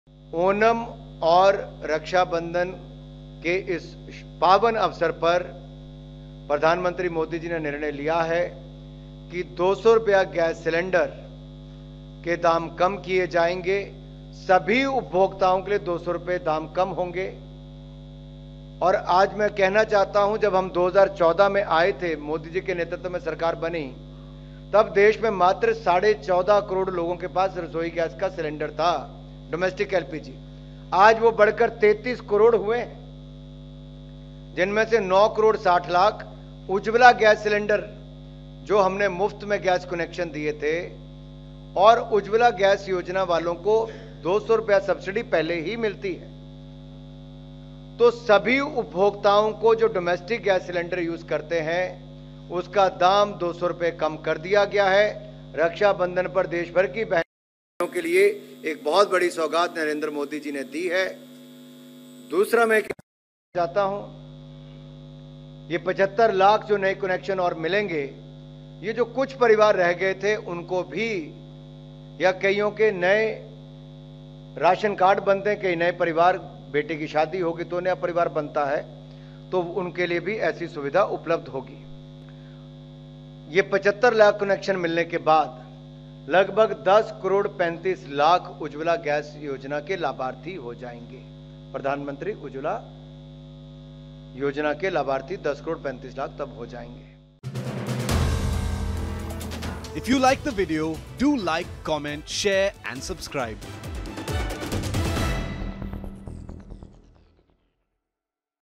Festive Delight: Gas Cylinder Prices Slashed By Rs 200 For Rakshabandhan And Onam
PM Modi has decided Rs 200 reduction in the price of domestic LPG cylinders, for all users...this is a gift from PM Narendra Modi, to the women of the country, during the occasion of Raksha Bandhan and Onam,” said Union Minister Anurag Thakur.